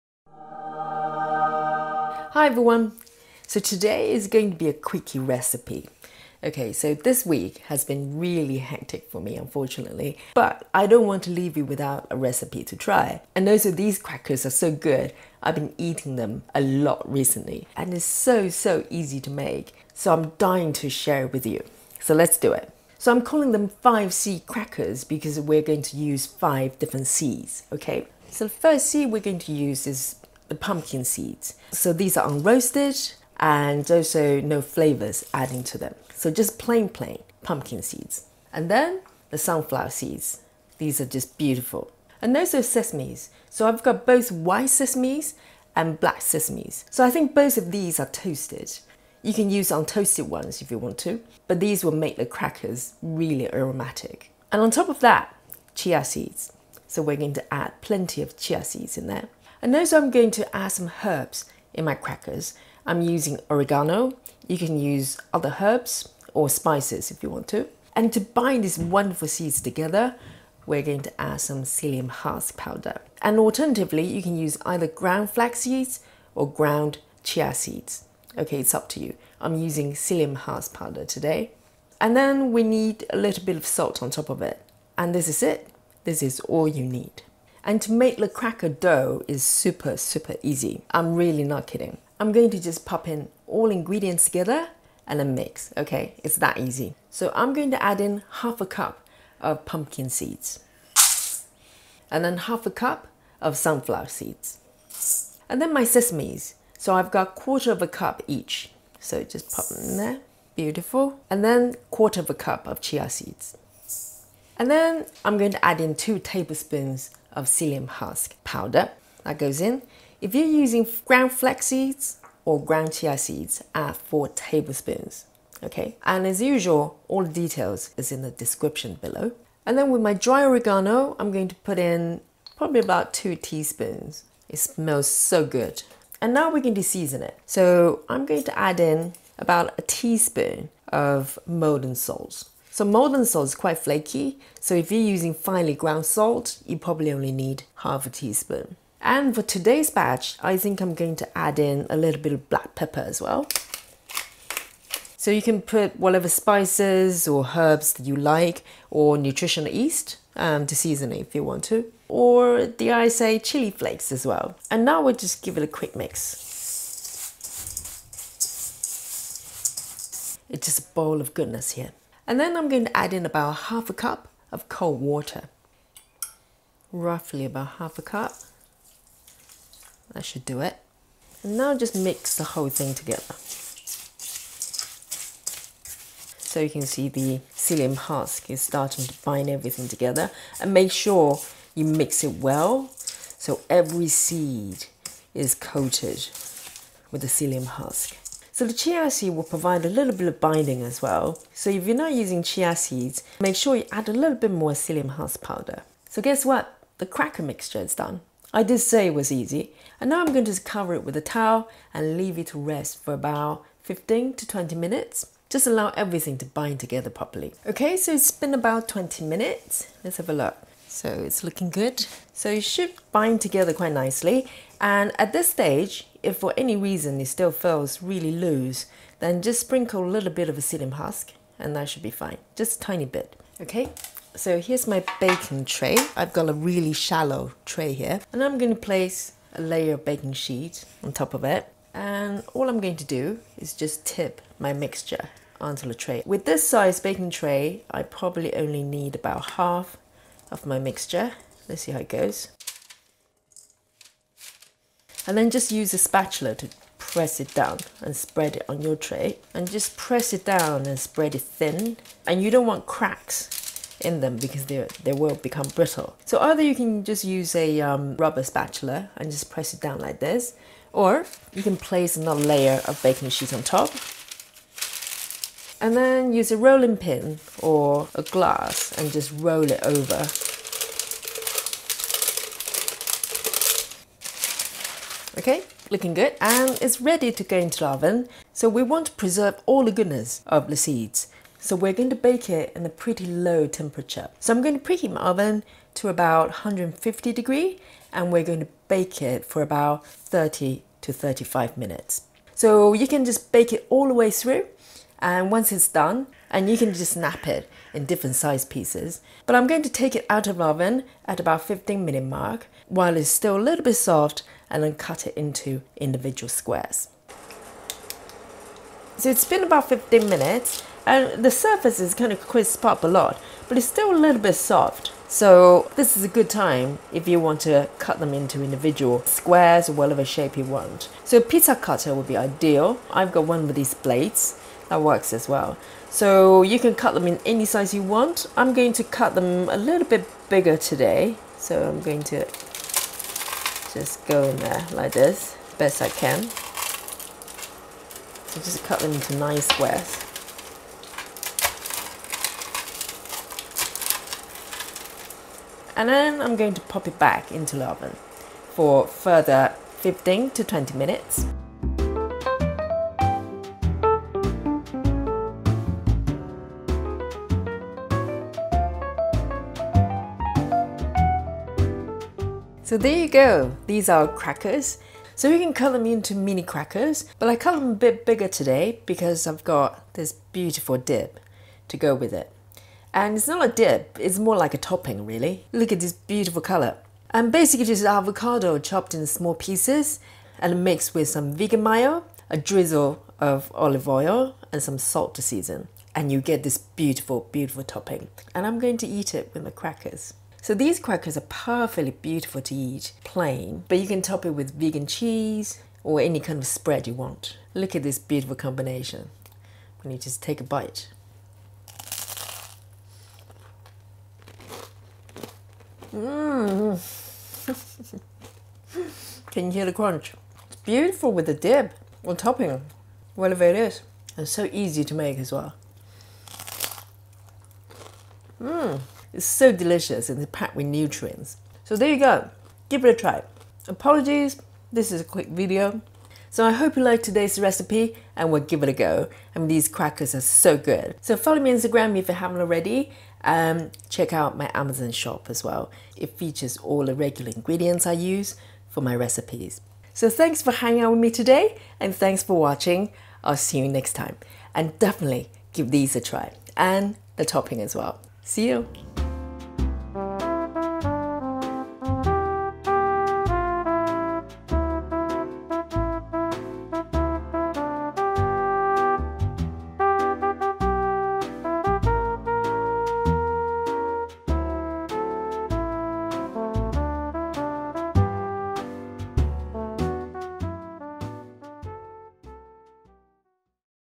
Super crispy keto 5 seeds crackers | keto vegan gluten-free
These crackers are wonderfully crispy and are packed with nutrients and wholesome goodness. Enjoy as the are or topped with vegan cheese and spreads. A great way to start a day or as a snack!

INGREDIENTS
1/2 cup pumpkin seeds
1/2 cup sunflower seeds
1/4 cup chia seeds
1/4 cup white sesames
1/4 cup black sesames
2 tbsp psyllium husk powder
(or 4 tbsp ground flaxseed)
(or 4 tbsp ground chia seed)

1 tsp coarse sea salt (adjust to your taste)
2 tsp dried oregano (or other herbs of your choice)
½ cup cold water

DIRECTIONS
1. Mix the seeds, psyllium husk powder, dried herb and salt together in a bowl.

2. Add in the water and mix everything well. Leave to rest for 15-20mins.

3. Spread the seed mixture on an ungreased baking tray (regular cracker thickness). Note: if the mixture still feels too loose at this stage, sprinkle a small amount of psyllium husk powder)

4. Bake in a 150C/300F preheated oven for 30-35 mins (or till crispy). Option to cut crackers half way into the baking time while the mixture is still slightly soft (10-15 mark).

5. Leave to cool for a few minutes and enjoy.

AVOCADO SPREAD
Chopped avocado
Vegan mayo (or vegan yoghurt)
A drizzle of extra virgin olive oil

Mix all ingredients together and that’s it!